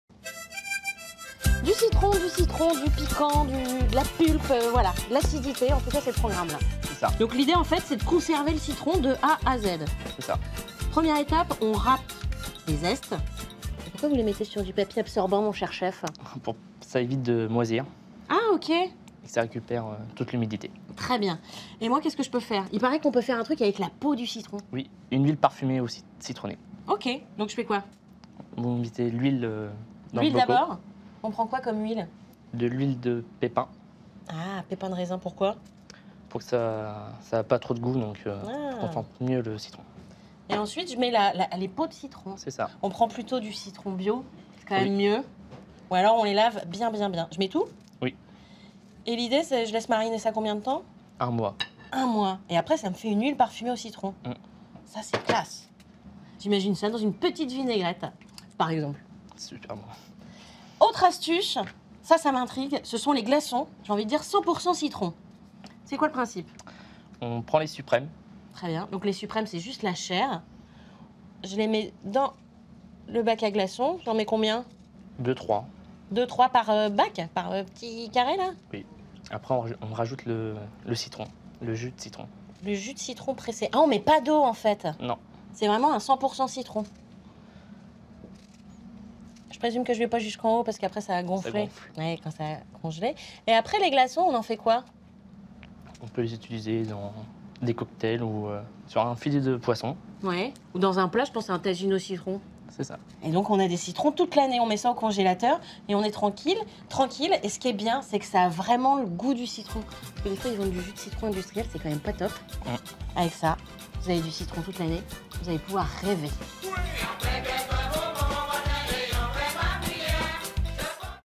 Astuces pour bien conserver ses citrons
Une recette d'Adrien Defrance, élève au lycée hôtelier Georges Baptiste de Canteleu (76)